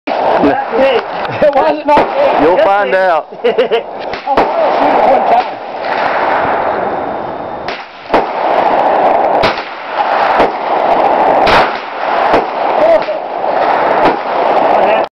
9MM SHELL EXPLODES IN CHAMBER OF MACK 11!!! MUST SEE!!
2nd shot explodes in chamber! Luckily noone gets hurt.